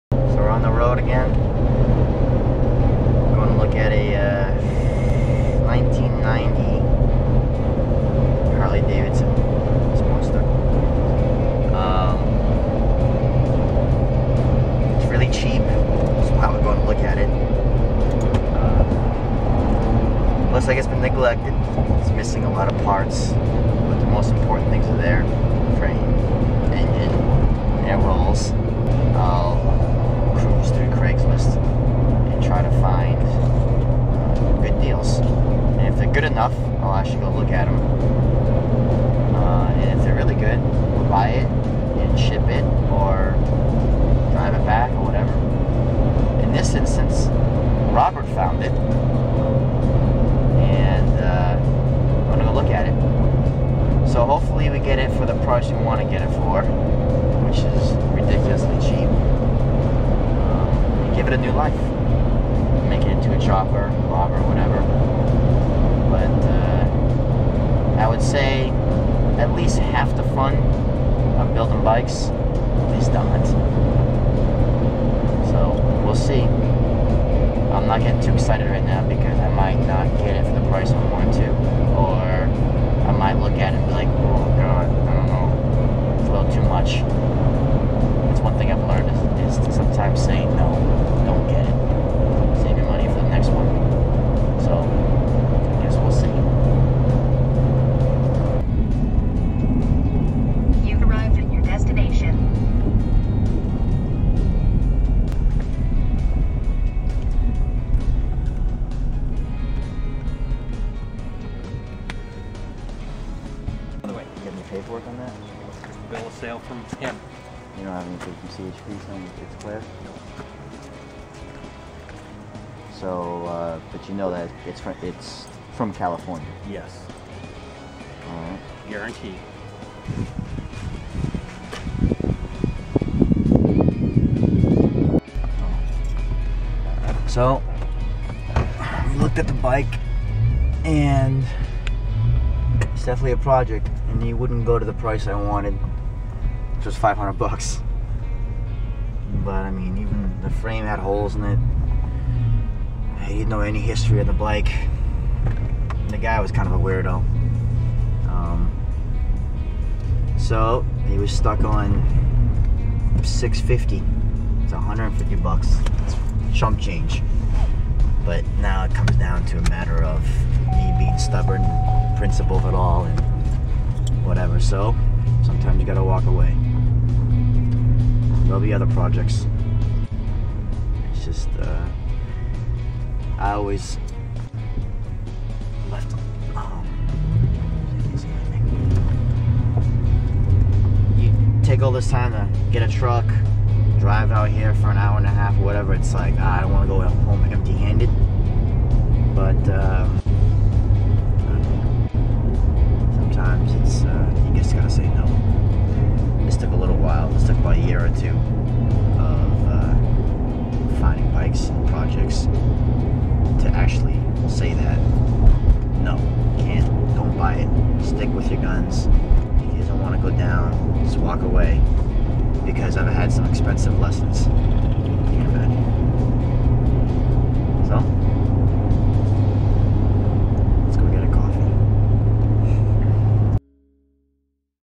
Bike Hunting
Going to check out a Sporty to see if its going to be the next build. Half the fun is the hunt.